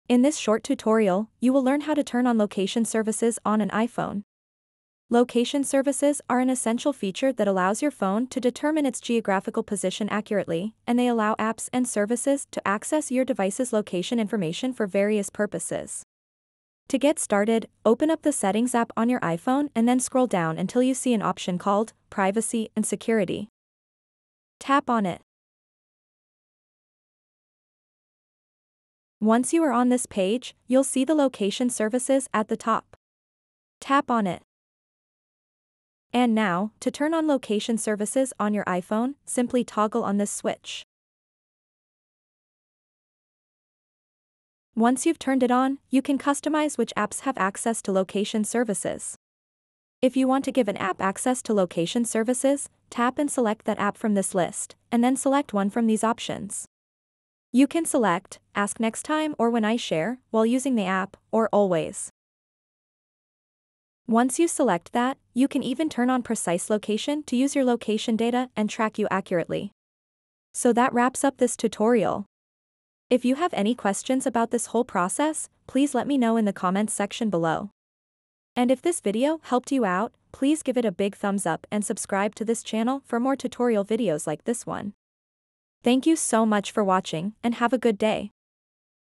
How To Turn On Location Services On iPhone
In this short tutorial, we will show you how to turn on location services on your iPhone. This feature can be really helpful in many situations. For example, if you want to find nearby restaurants, location services can help you by showing you a list of places to eat that are close to your current location. It's also useful for getting accurate weather forecasts based on where you are. So, no matter where you go, you can always be prepared for the weather. Additionally, turning on location services enables you to use navigation apps effectively, making it easier for you to find your way around in unfamiliar places. With just a few simple steps, we'll guide you through the process of turning on location services on your iPhone so that you can take full advantage of its benefits!